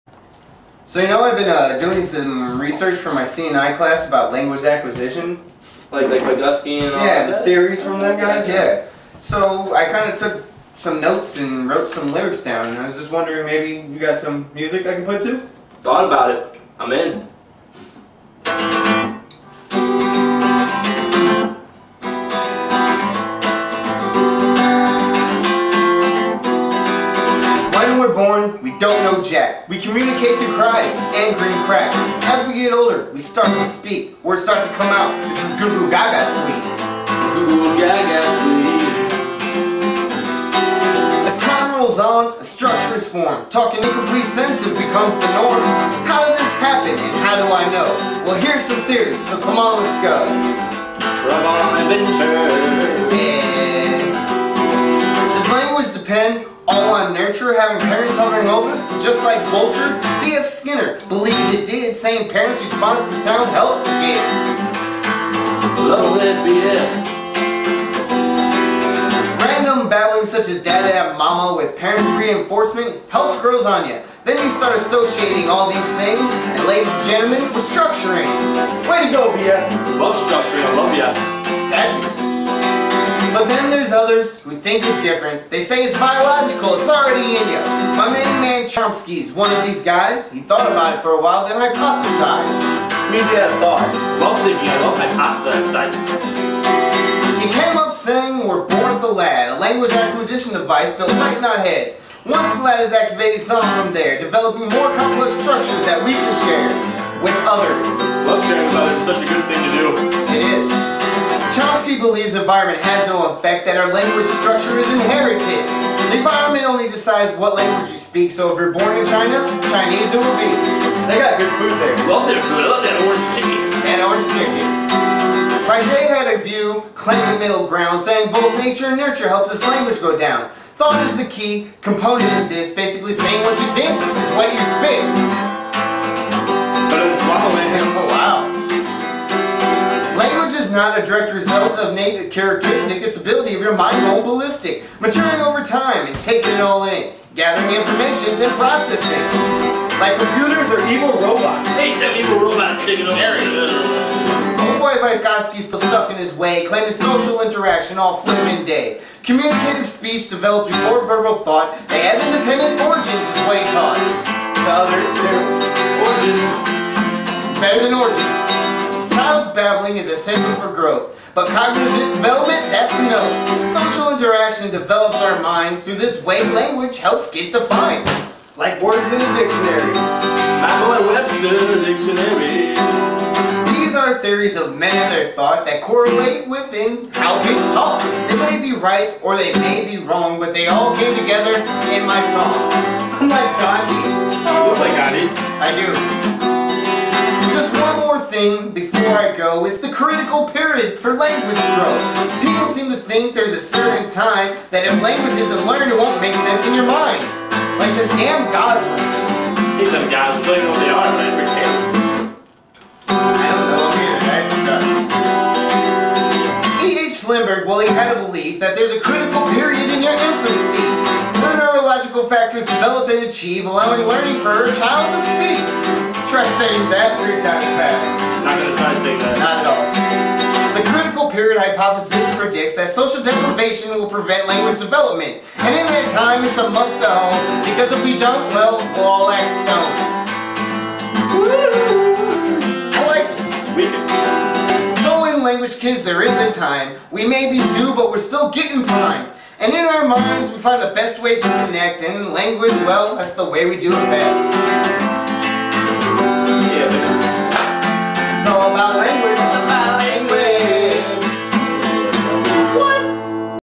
Language Development.mp4
Robert Kawczynski and Ryan Patterson performing a song about language development for a class at Illinois State University.